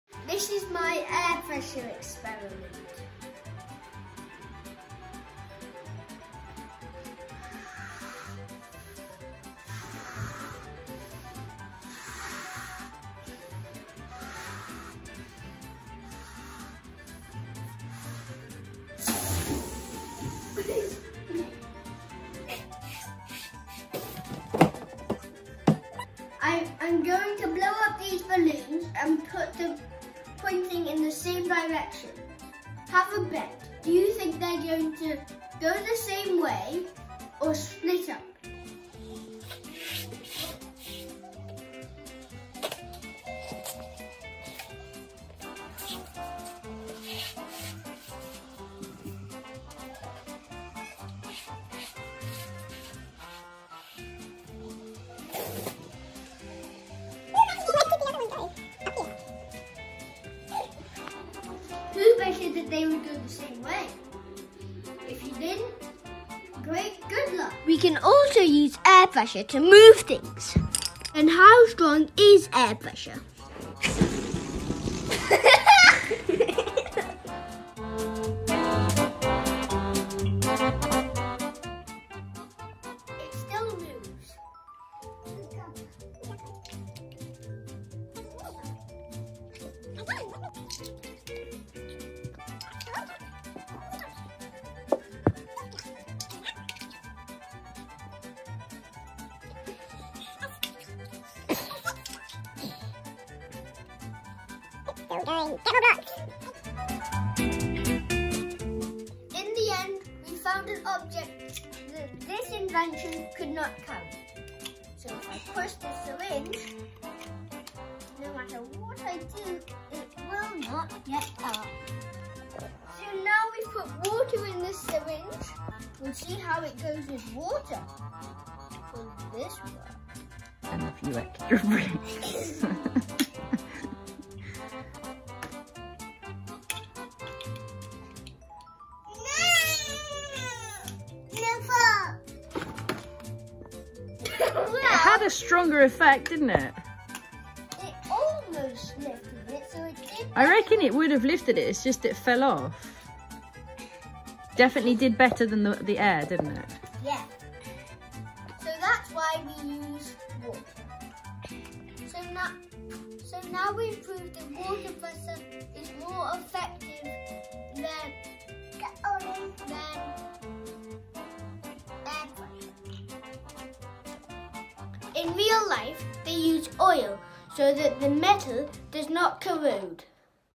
Water and air pressure experiments
Using MEL science and kiwi crates and balloons